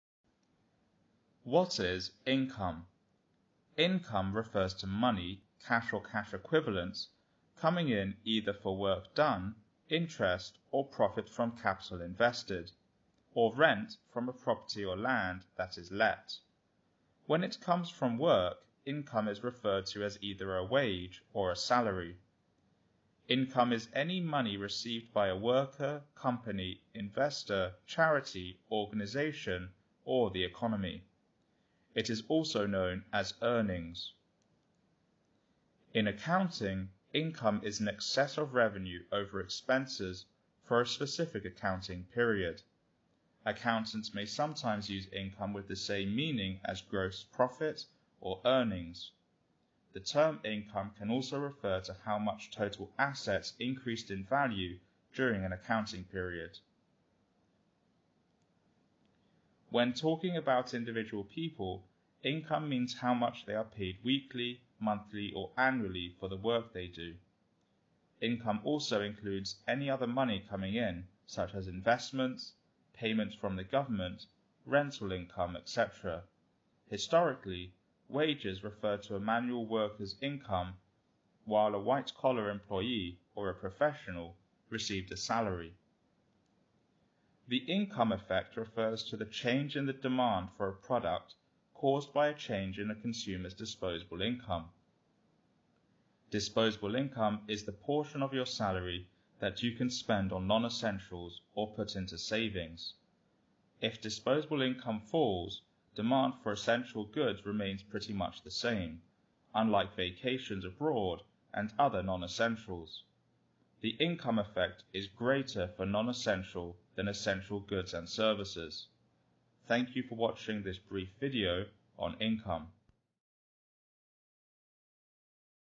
What is income? Definition and meaning
This video explains what income is. Put simply, income refers to money – cash or cash-equivalents – coming in either for work done, interest or profit from capital invested, or rent from a property or land that is let.